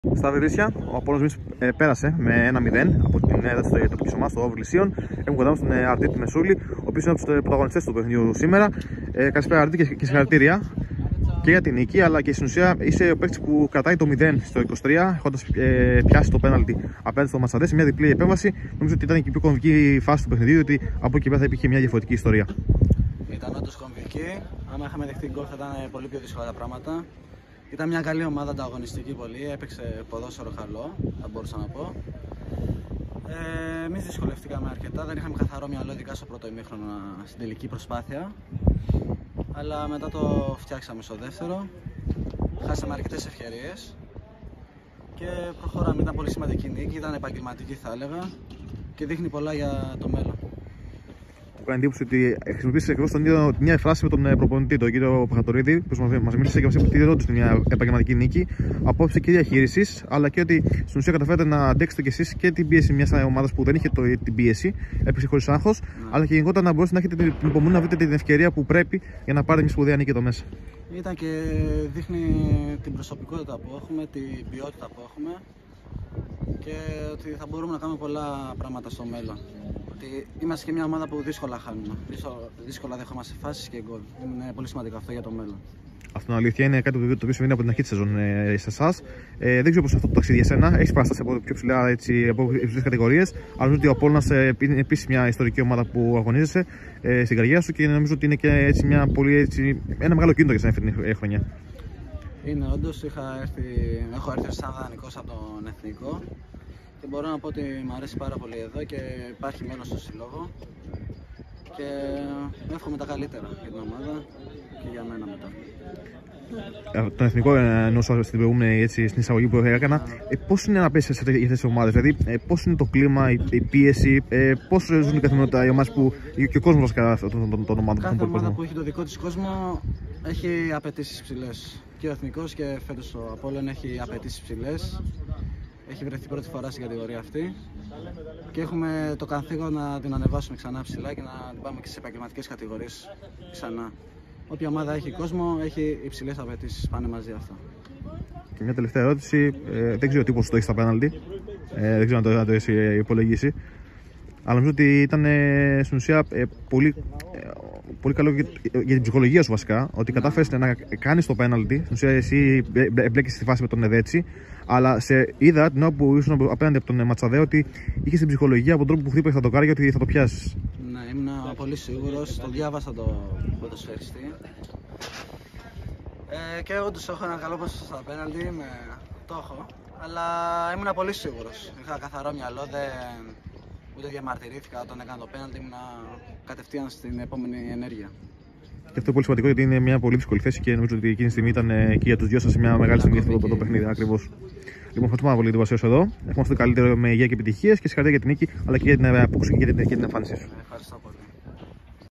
Ο Αρντίτ Μεσούλι για τη νίκη του Απόλλωνα στα Βριλήσσια, τα πέναλτι και η πίεση στις μεγάλες ομάδες
Ρεπορτάζ: Γιώργος Γραμματικός